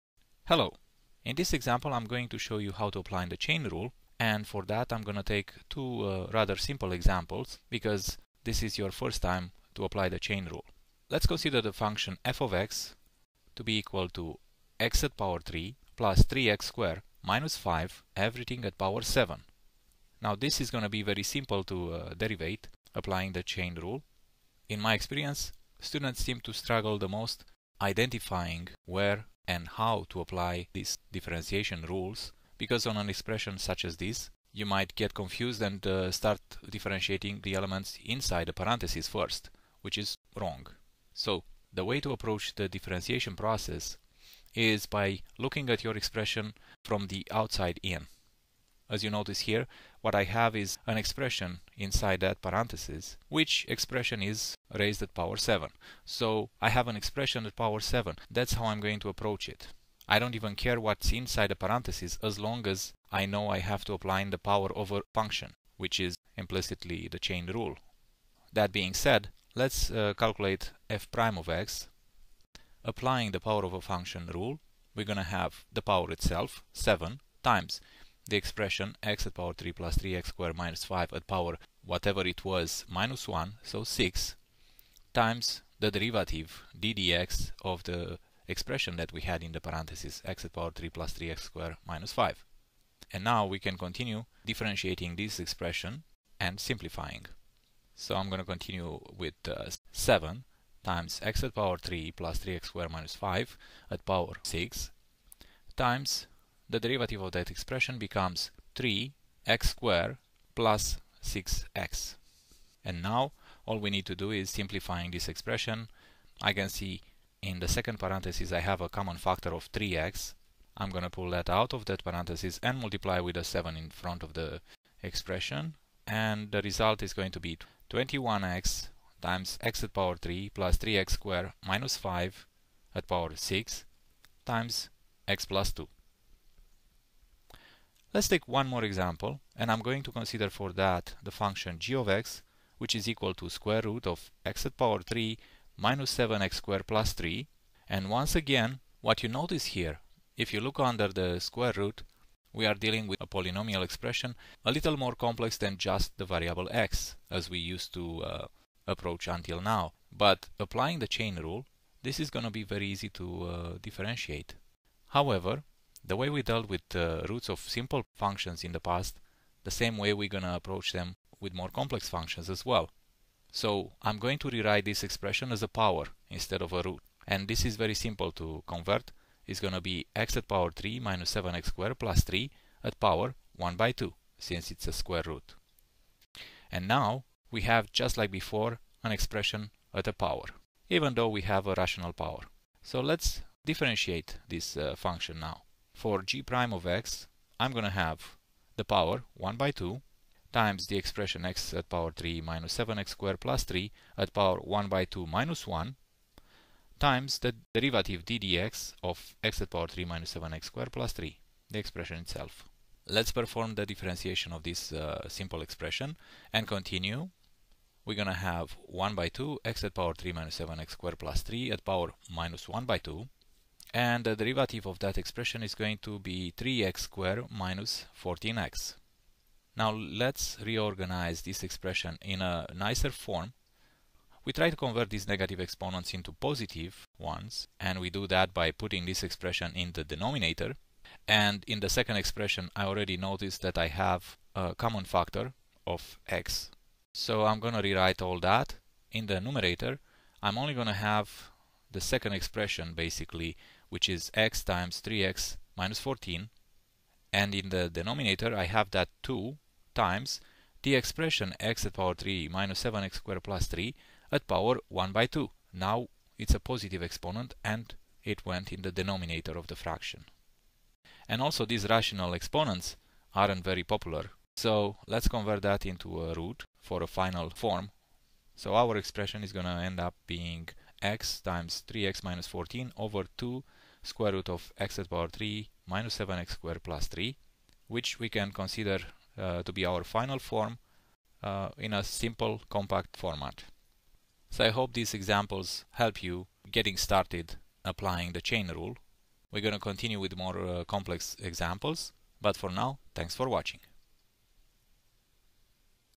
2.4. Chain Rule - Example1
This example applies the chain rule on a couple of functions, demonstrating the way you should start when dealing with functions that are more complex for any of the previous rules.